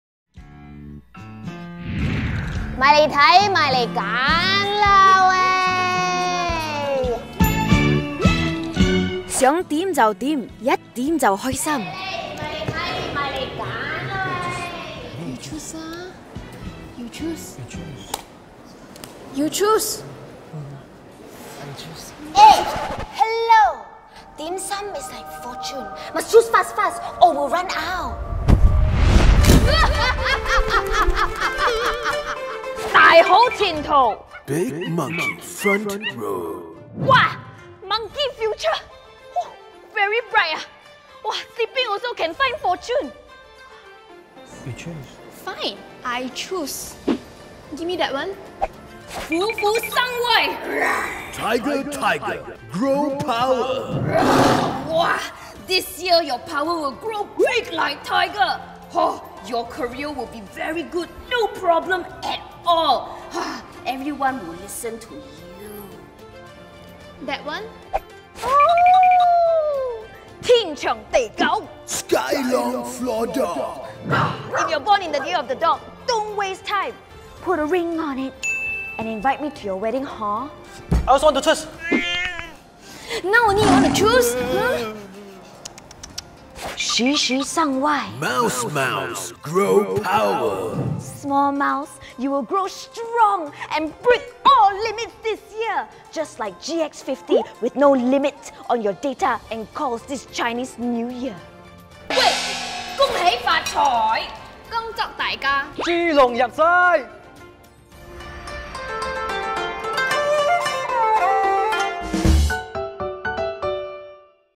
Choose What Your Heart Desires: Part 2 - U Mobile 2019
Born in the Year of the Monkey, Tiger, Dog or Rat? Don't miss the giler predictions* for your Chinese zodiac this year!